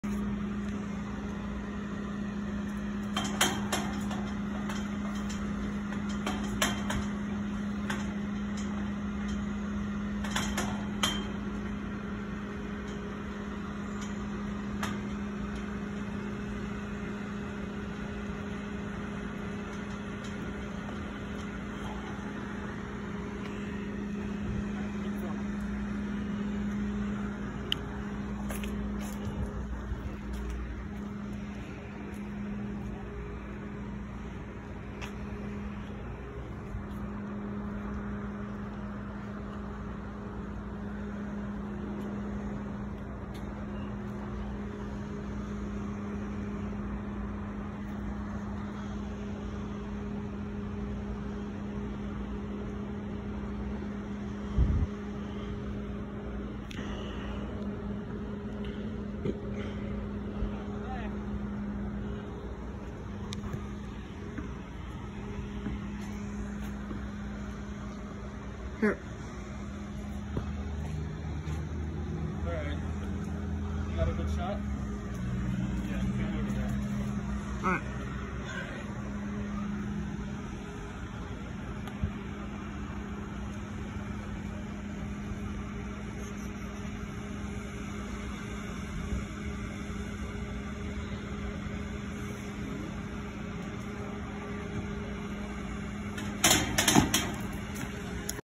Climbing school roof
I do this a lot play catch with us at lunch where over by the cafeteria